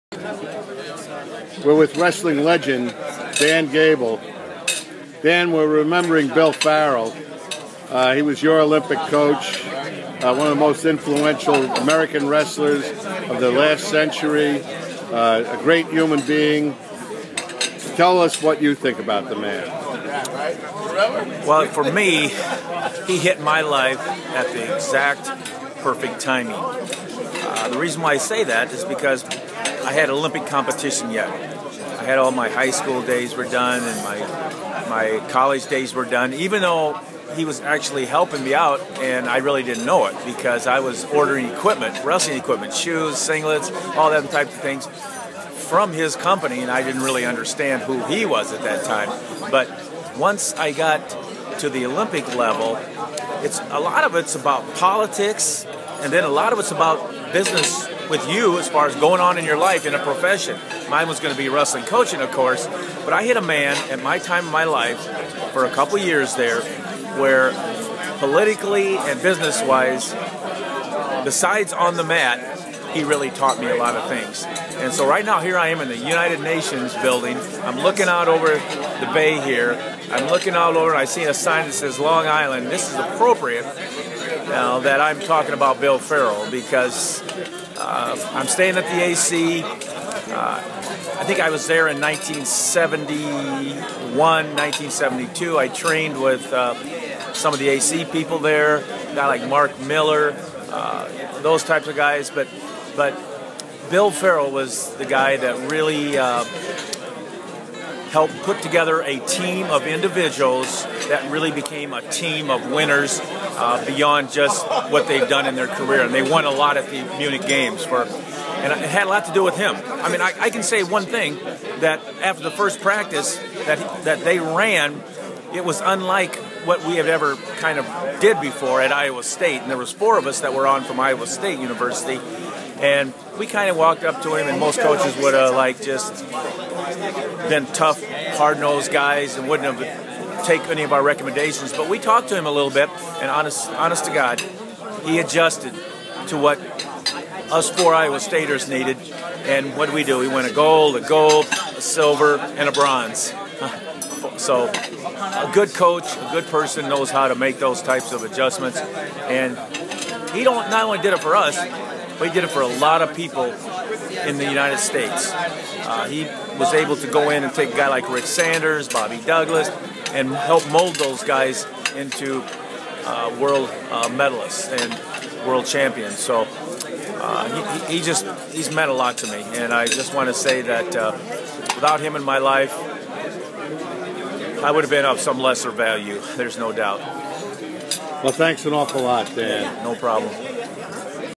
Dan Gable talks about his Olympic Coach
Bill Farrell was that and much more